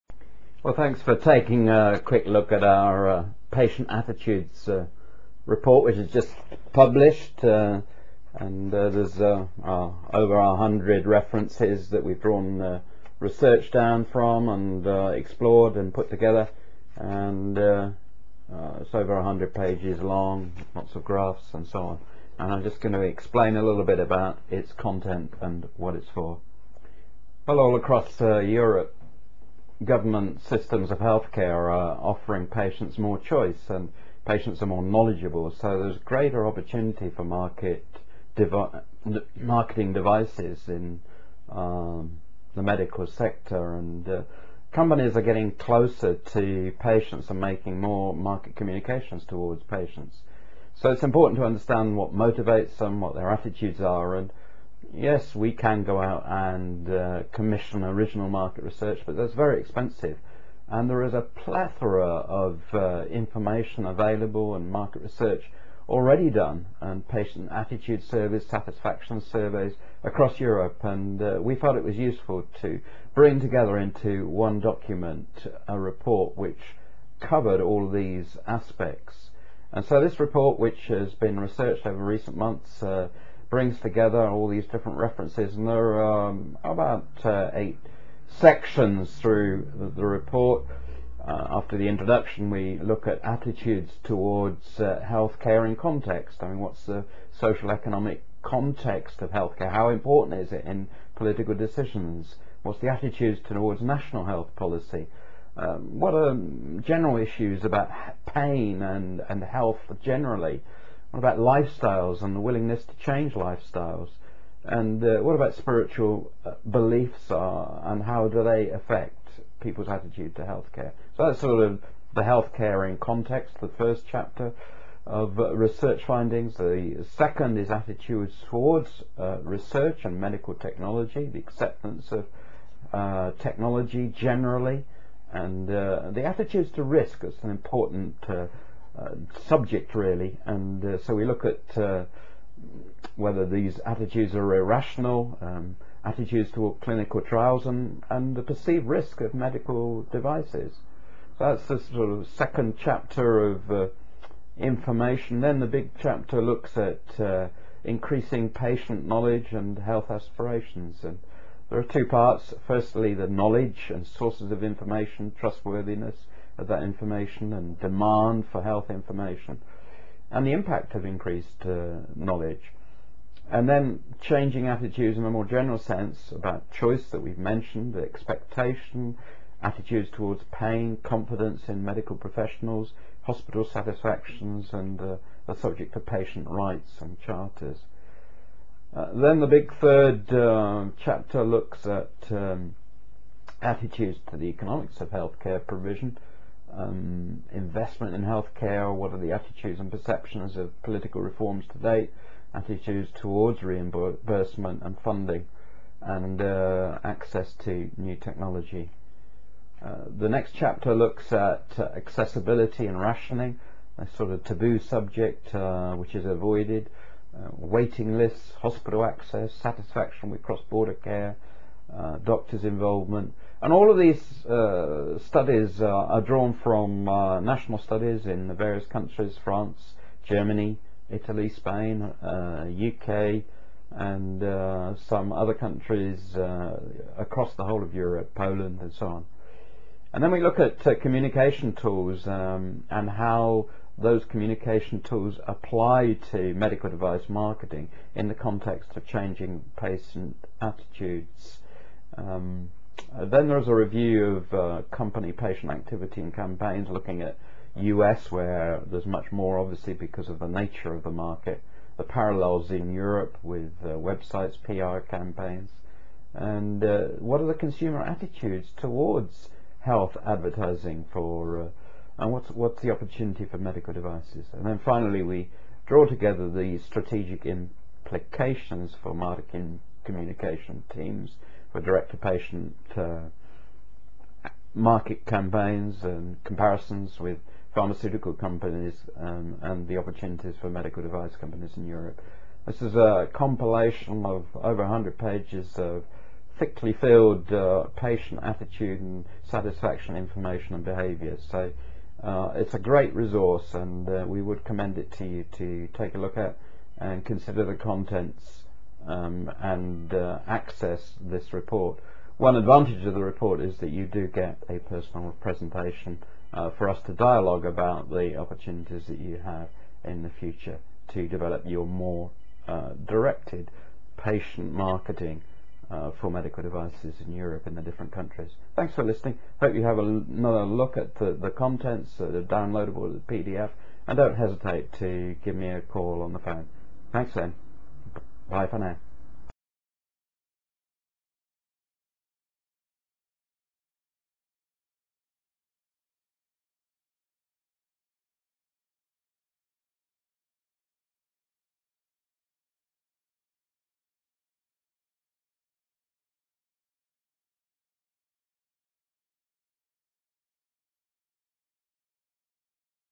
Patient attitudes and medical device marketing Europe
Introduction to TG2 research project: 100 pages, powerpoint charts for 73 figures and 24 tables